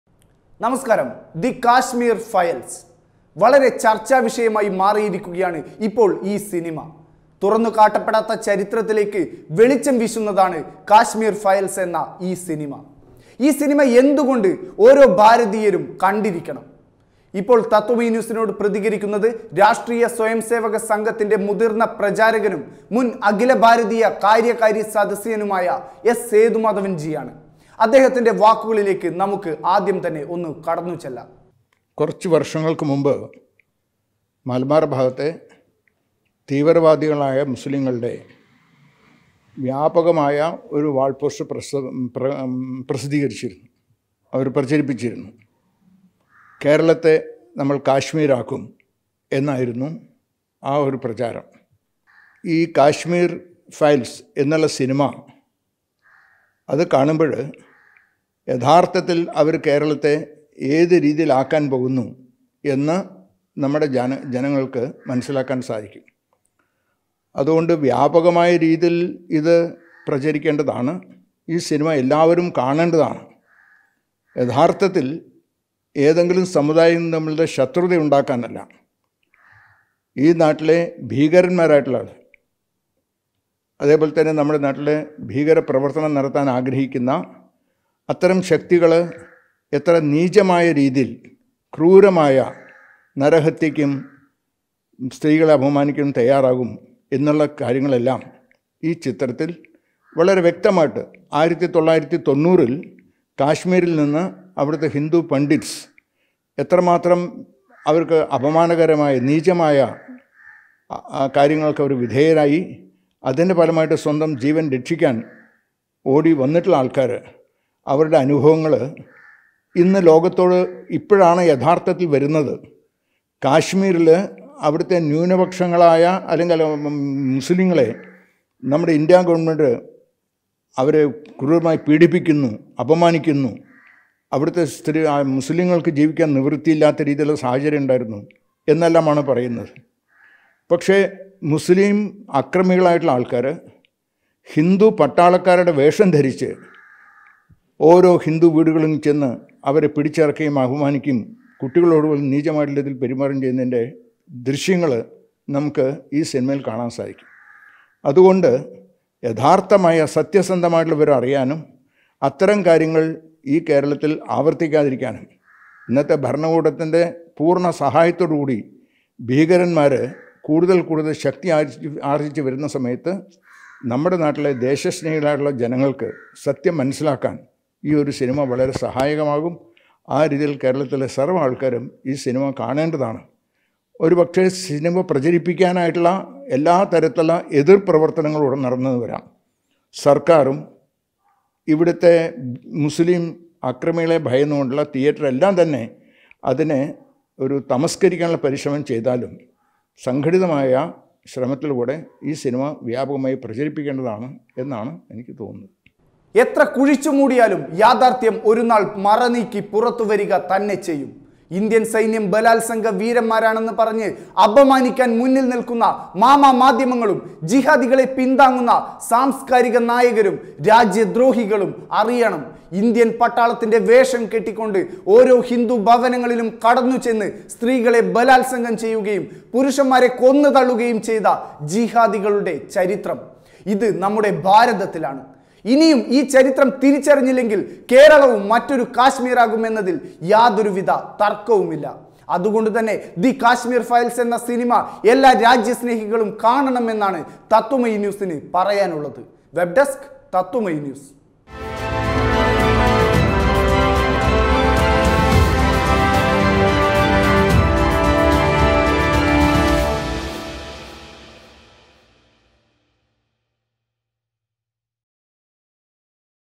ഇത് കഥയല്ല ഞെട്ടിപ്പിക്കുന്ന ചരിത്രം, എസ് സേതുമാധവൻ ജി പ്രതികരിക്കുന്നു | The Kashmir Files Movie
ഇന്ത്യൻ പട്ടാളത്തിന്റെ വേഷം കെട്ടി ഹിന്ദു സ്ത്രീകളെ ബലാത്സംഗം ചെയ്ത ജിഹാദികളുടെ ഞെട്ടിക്കുന്ന ചരിത്രം | The Kashmir Files Movie